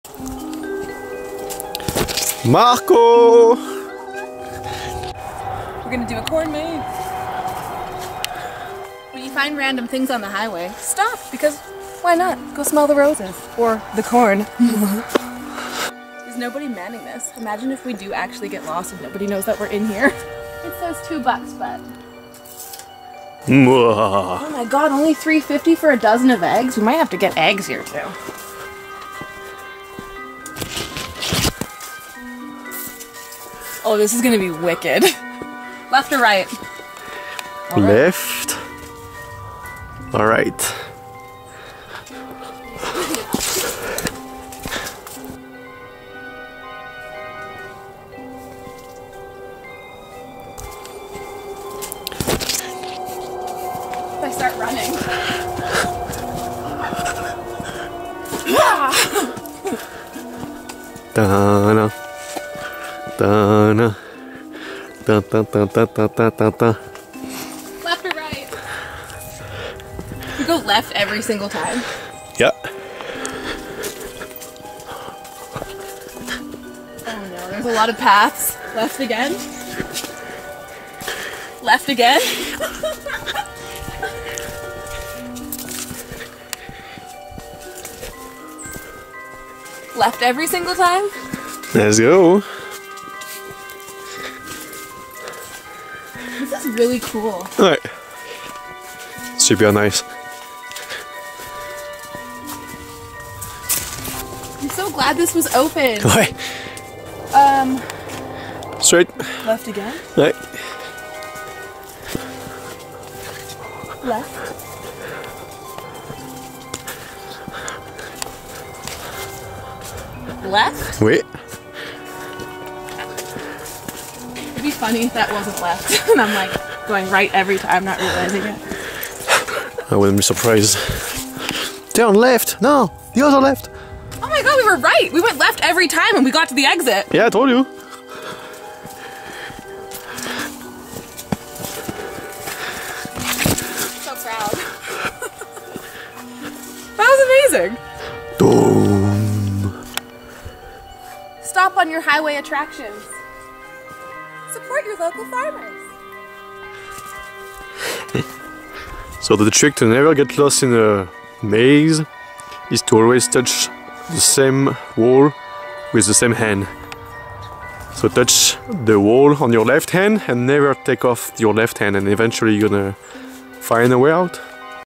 🌽 Explore A Corn Maze With Us!
My first time in a Corn Maze!!!
I have always wanted to do this.

Today: Cariboos Corn Maze
Located South of Quesnel, BC CANADA

We do not claim to be professionals, by any means.
Just 2 semi-normal humans, from differant sides of the world, on random outdoor adventures!

If you like our content and want to support us...

Max & Jen

~ Sony DSC-HX400V
~ Samsung Galaxy A14
~ Lenovo Laptop